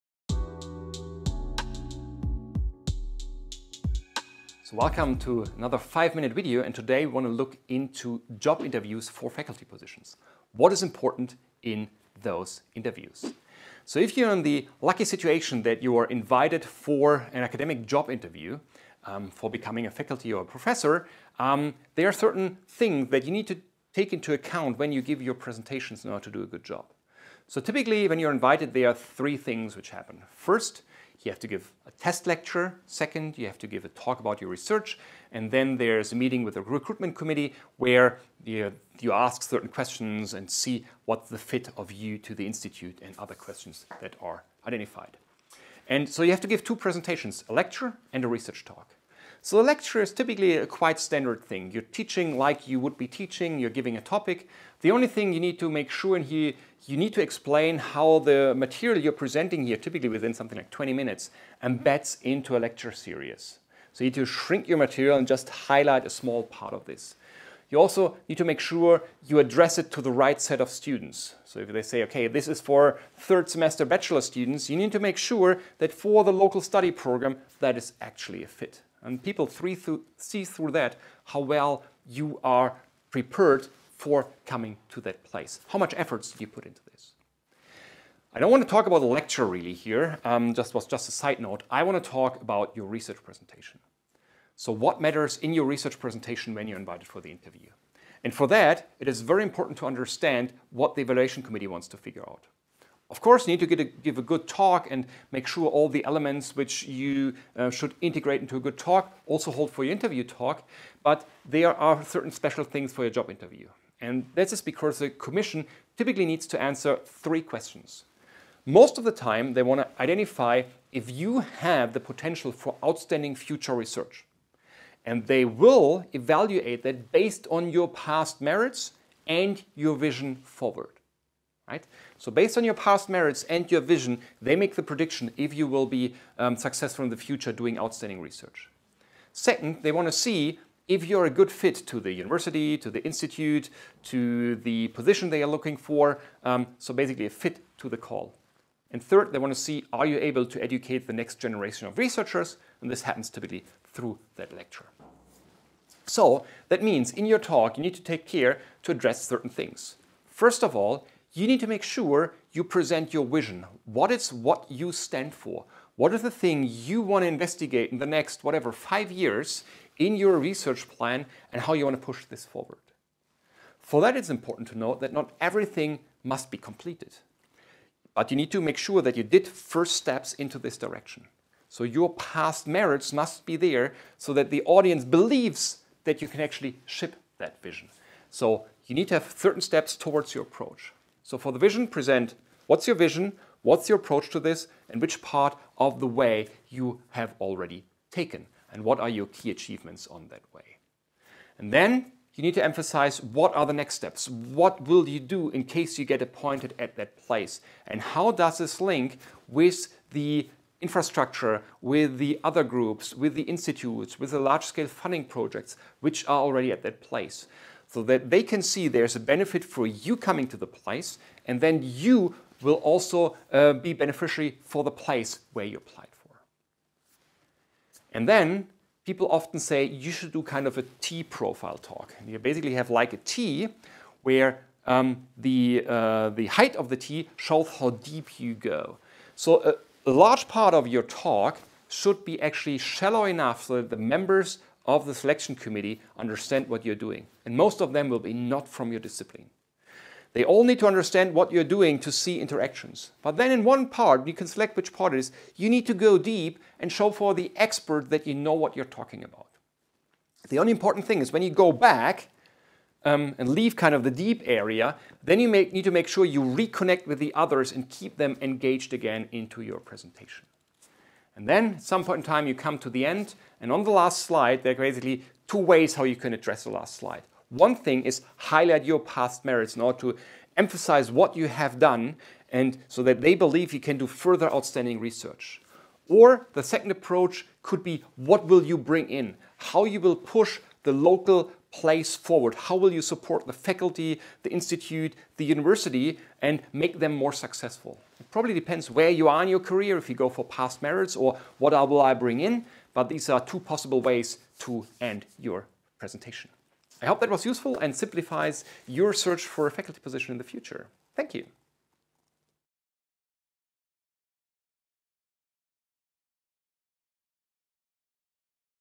Academic Job Interview Talks - 5 Minutes with Cyrill
What matters for your talk in an academic job interview explained in 5 minutes
Series: 5 Minutes with Cyrill
Cyrill Stachniss, 2022

Credits:
Video by Cyrill Stachniss
Special thanks to Olga Vysotska and Igor Bogoslavskyi